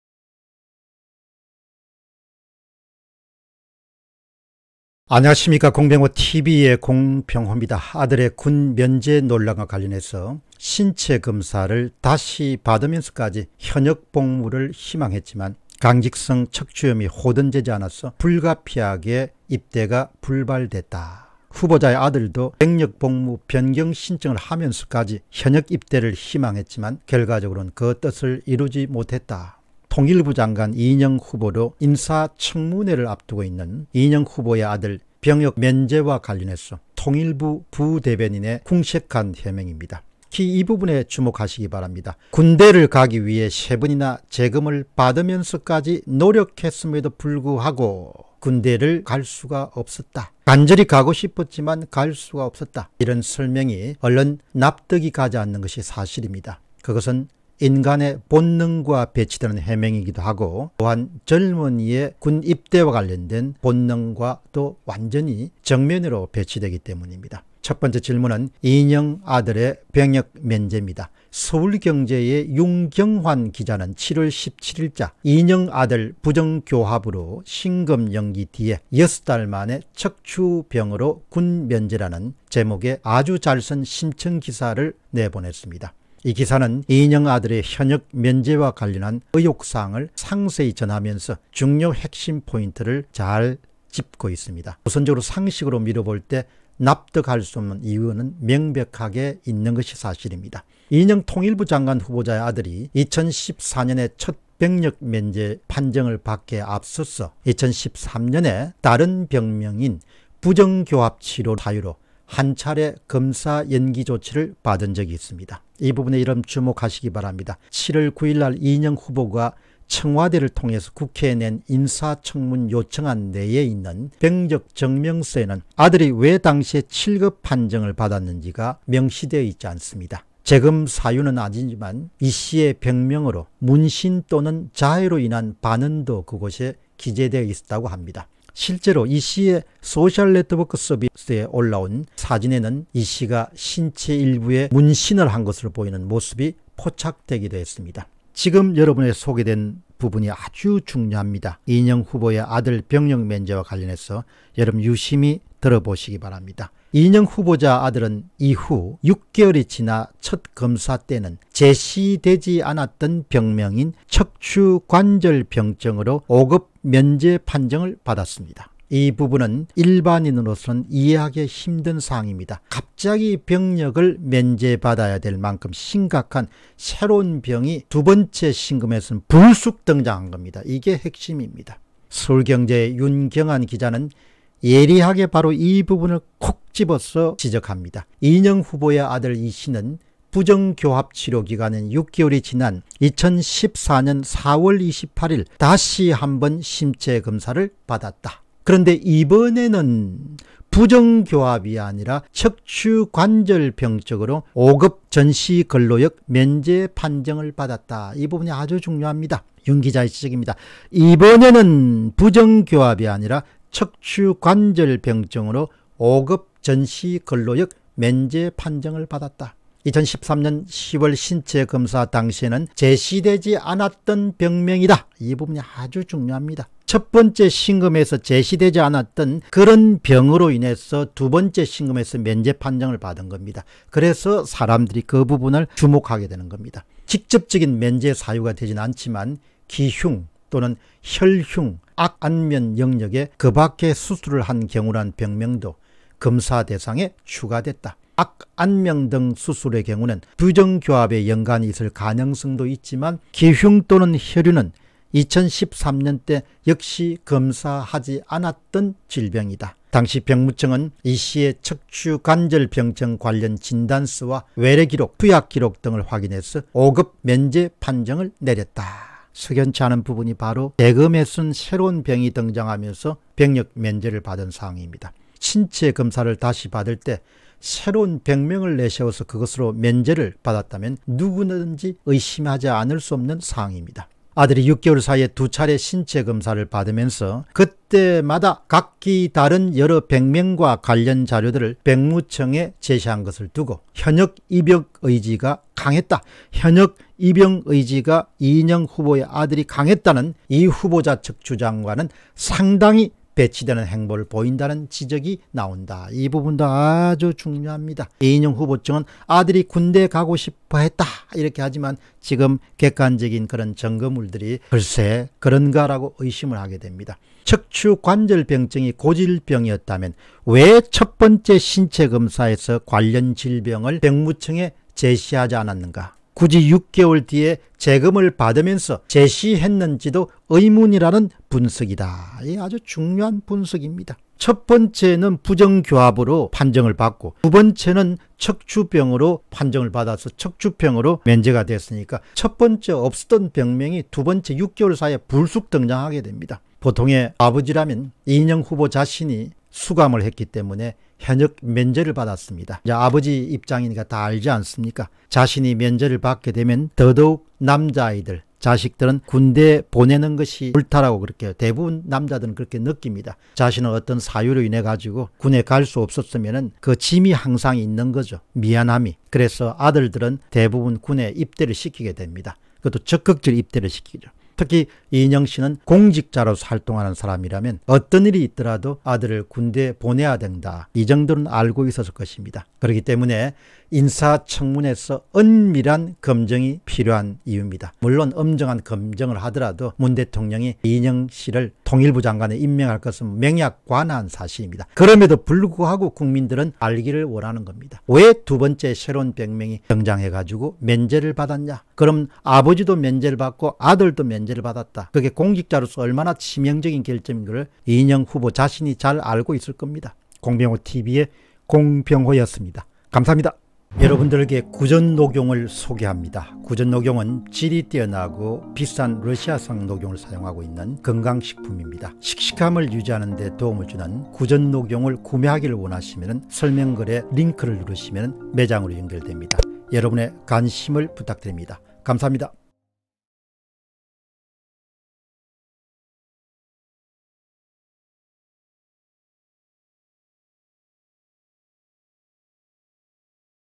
이인영 아들, 병역면제 / 석연치 않다 [공병호TV]
Bitcoin : 3MTW849AkP2ftLeSK2teZHMmzLu7Z83s9U

참고자료
1. 이인영 측 "아들, 군대 가려고 재검까지 받았지만…척추염 탓에 '불발'", 뉴스핌, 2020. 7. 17
2. [단독] 이인영 아들, '부정교합'으로 신검 연기 뒤 6달만에 '척추병'으로 軍면제, 윤경환, 서울경제, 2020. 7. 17